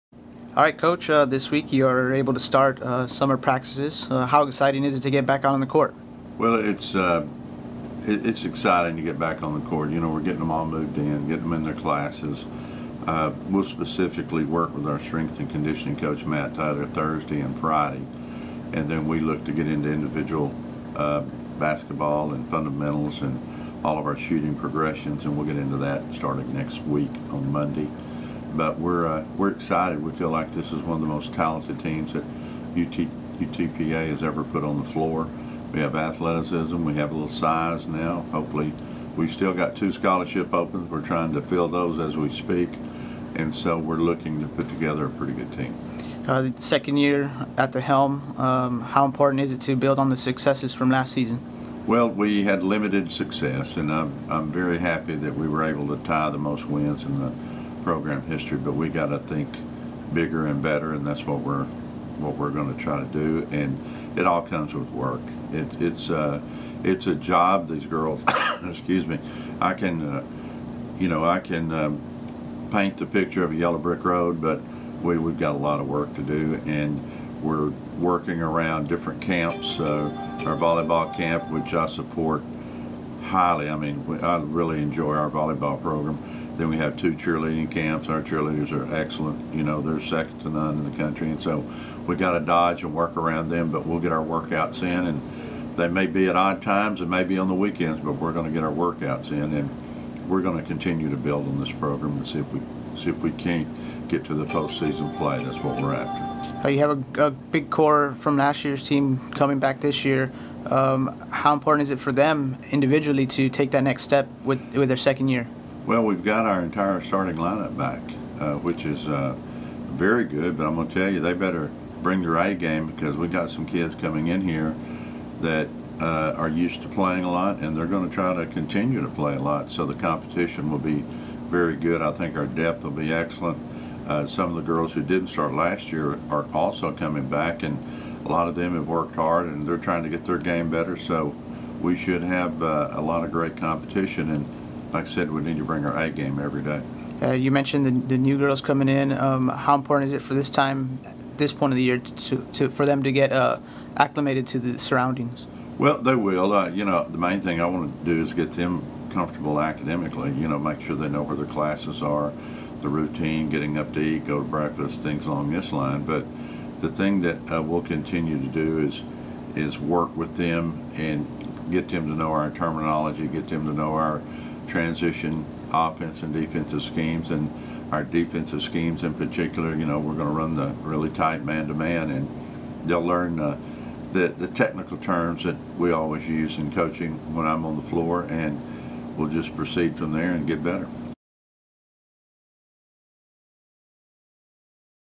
Women's Basketball Set For Summer Practice
EDINBURG -- The University of Texas-Pan American Broncs women's basketball team is ready to kick off their 2014-15 season as they prepare for their summer workouts and practices this week.

Broncs head coach Larry Tidwell is not only excited to get back on the court but he is excited to see his student-athletes back in the classroom.

"It is exciting to get back on the court," Tidwell said. "We are getting them all moved in and getting them in their classes. We are excited. We feel like this one of the most talented teams that UTPA has ever put on the floor. We have athleticism and a little size. We are looking to put together a pretty good team."

Heading into the second season under Tidwell, the Broncs look to build on their success from last season.

"We had limited success," Tidwell said. "I am very happy that we were able to tie the most wins in program history but we have to think bigger and better. That is what we are trying to do. It all comes down to work."

The Broncs return three of their top four scorers from last year in Shawnte' Goff, KaeLynn Boyd and Brittany Bush. Coach Tidwell hopes that another year in his system allows them to continue to improve.

"We are going to continue to build this program and see if we can get to postseason play," Tidwell said. "We have our entire starting lineup back, which is very good. The competition will be very good and the depth will be excellent. We should have a lot of great competition."

Coach Tidwell plans on using his time wisely this summer. While the Broncs have most of last season's team returning, they have four new faces that will be getting their first taste of college basketball.

"The main thing I wanted to do was to get them comfortable academically," Tidwell said. "I wanted them to make sure they knew where their classes are. The thing that I will continue to do is to work with them and get them to know our terminology. We will proceed from them and get better."